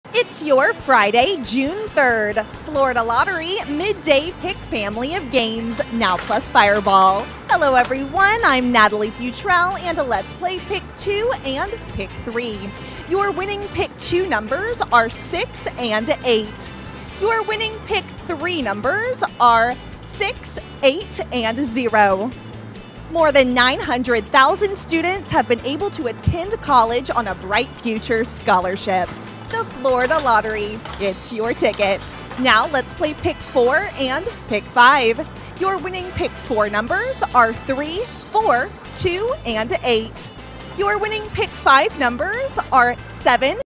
PICK3 AND PICK4 DIA Resultado del VIERNES 03 DE JUNIO de 2022 ✅🥇🔥💰
Hola buenas DIAs, te presentamos los resultados de las loterías y chances en Colombia, vamos con el Resultados del Chance El PICK3 AND PICK4 DIA del VIERNES 03 DE JUNIO de 2022.

Resultado Loteria de Medellín VIERNES 03 DE JUNIO de 2022 (Número Ganador) 💰💰💰Resultados Lotería de Medellín 03 DE JUNIO de 2022